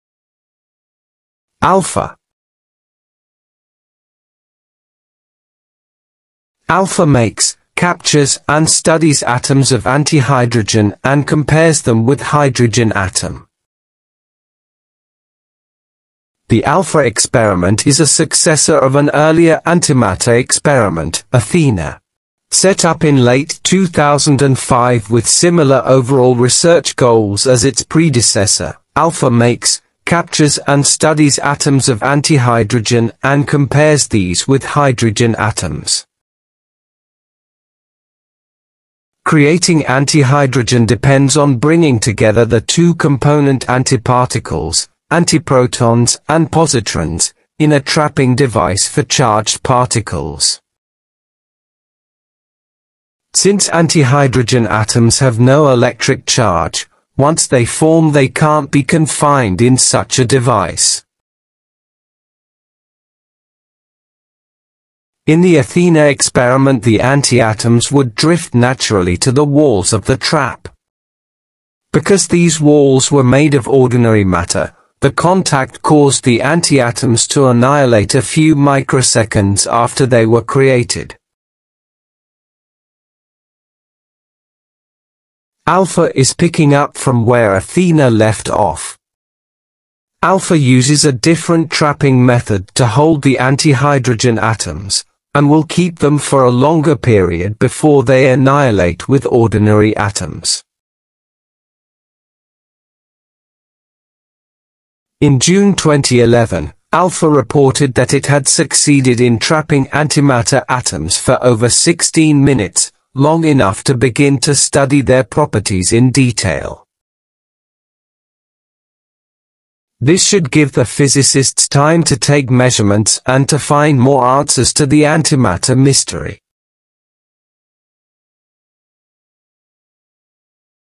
ALPHA - (Antihydrogen Laser Physics Apparatus) Experiment - CERN
In this video, we take a fascinating journey into the world of antimatter as we explore the cutting-edge ALPHA experiment at CERN's Antihydrogen Laser Physics Apparatus (ALPHA). Join us as we discover how scientists are using lasers to capture and study antihydrogen atoms, and learn about the groundbreaking discoveries that could change our understanding of the universe.

AEgIS - CERN |  Antihydrogen Experiment: Gravity, Interferometry, Spectroscopy (AEgIS) |

Anti Matter

Z BOSON

W BOSON

Higgs Boson

Part 2